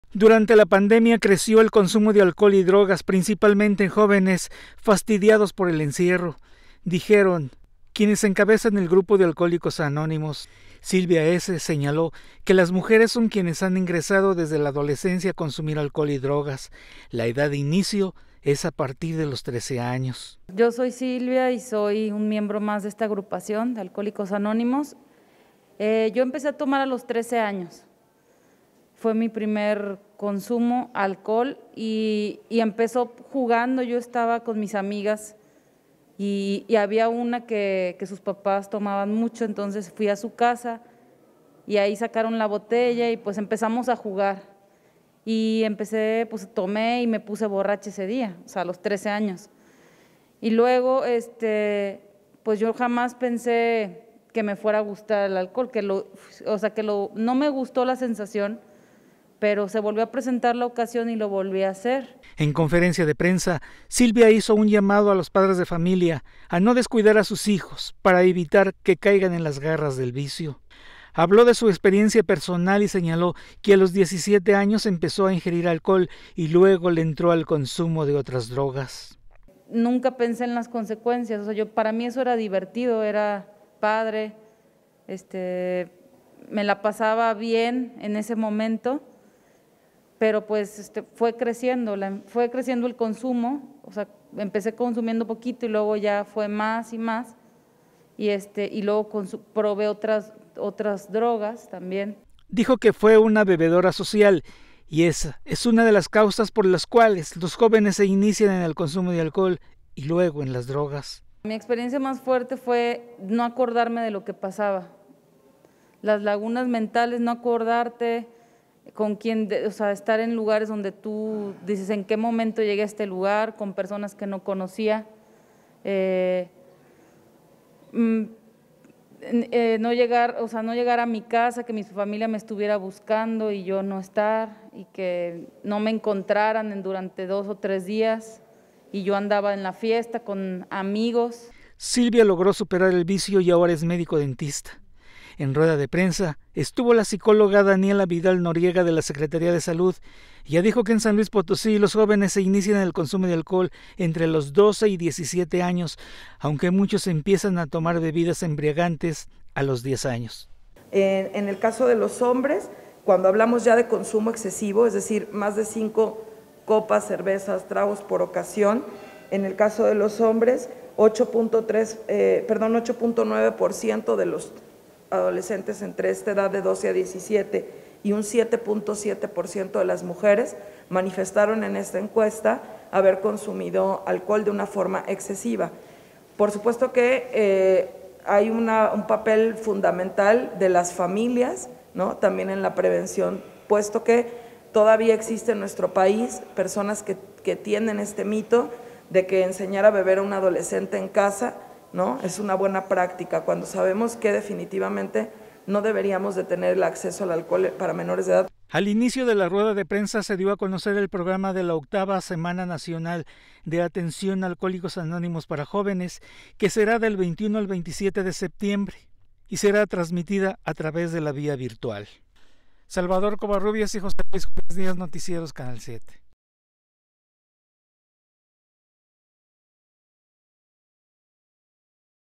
Aumenta consumo de alcohol en menores de edad durante la pandemia: AA.
SLP.- Durante la pandemia creció el consumo de alcohol y drogas, principalmente en jóvenes fastidiados con el encierro, dijeron representantes de un grupo de Alcohólicos Anónimos.

Silvia S. señaló que las mujeres son quienes han ingresado desde la adolescencia a consumir alcohol y drogas. La edad de inicio es desde los 13 años.

En conferencia de prensa, Silvia hizo un llamado a los padres de familia a no descuidar a sus hijos para evitar que caigan en las garras del vicio.

Habló de su experiencia personal y señaló que a los 17 años empezó a ingerir alcohol y luego le entró al consumo de otras drogas.

Dijo que fue una bebedora social y ese una de las causas por las cuales los jóvenes se inician en el consumo de alcohol y las drogas.

Silvia logró superar el vicio y ahora en médico dentista.

En la rueda de prensa estuvo la psicóloga Daniela Vidal Noriega, de la Secretaría de Salud dijo que en San Luis Potosí, los jóvenes se inician el consumo de alcohol entre los 12 y 17 años, aunque muchos empiezan a tomar bebidas embriagantes a los 10 años de edad.

Al inicio de la conferencia de prensa se dio a conocer el programa de la Octava Semana Nacional de AA para los jóvenes que será del 21 al 27 de septiembre. Eventos que se transmitirán a través de la vía virtual.